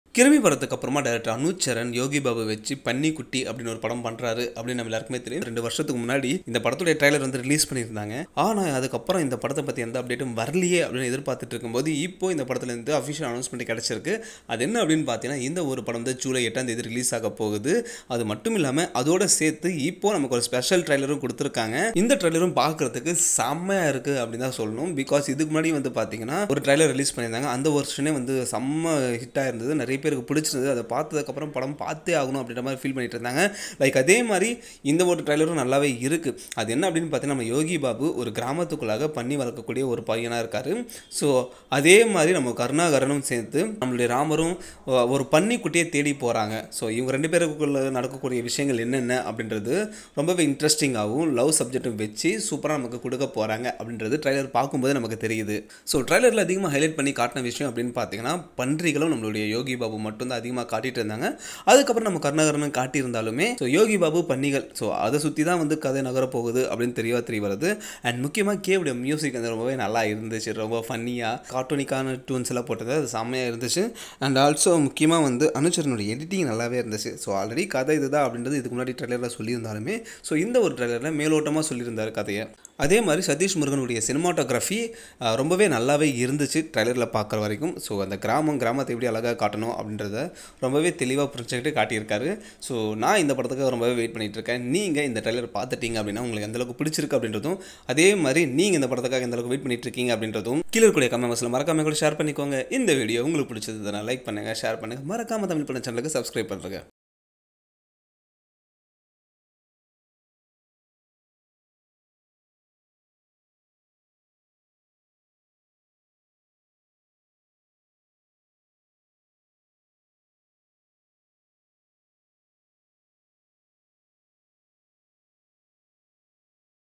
Panni Kutti Movie Trailer Review | Yogi Babu | Karunakaran | Lyca Productions | Thamizh Padam
Panni Kutti Movie Trailer Review on Thamizh Padam. Panni Kutti is a Tamil comedy movie directed and edited by Anucharan Murugaiyan, the movie is produced by Subaskaran under the banner Lyca Productions. The movie stars Yogi Babu and Karunakaran in the lead roles. The technical crew of Panni Kutty (aka) Panni Kutti includes Sathish Murugan for cinematography and K for music.